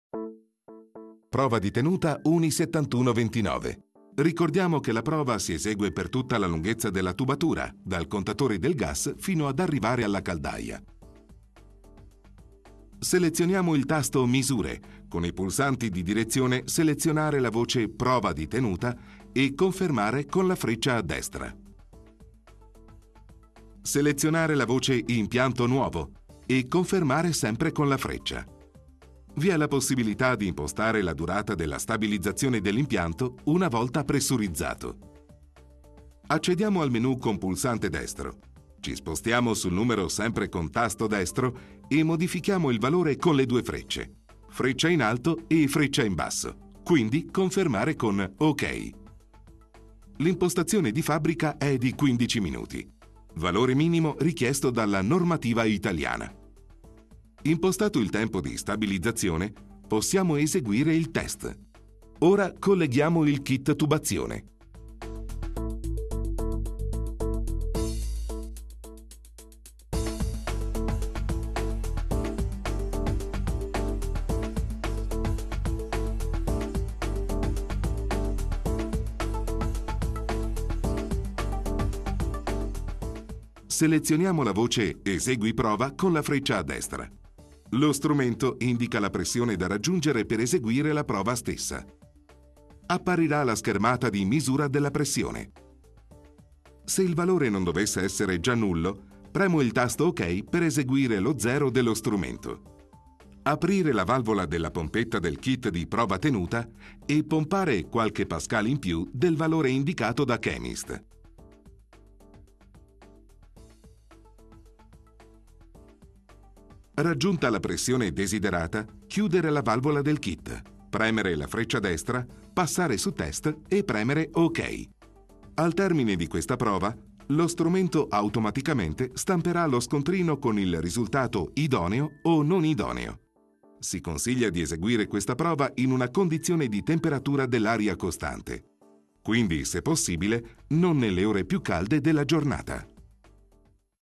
Analizzatore di combustione Chemist 400 - Prova di tenuta UNI 7129
Analizzatore di combustione con 2/3 sensori . Con ampio display grafico retroilluminato, stampante ad impatto integrata a carta comune, robusto pacco batterie ricaricabili, sensori precalibrati, sostituibili direttamente
dal'utente. Analisi dirette di O2, CO. Misure ausiliarie e calcolate: CO2, temperatura aria, fumi e differenziale, pressione, tiraggio, rendimento di condensazione secondo UNI 10389-1, eccesso d'aria. Prove di tenuta tubazioni secondo UNI7129 e UNI11137.
Fornito in valigetta rigida di plastica con alimentatore, sonda remota temperatura aria, sonda prelievo fumi da 180mm, gruppo filtro anticondensa/pulviscolo, tubo raccordato per misure di pressione, Manuale d'Istruzioni, software per PC e Certificato di Taratura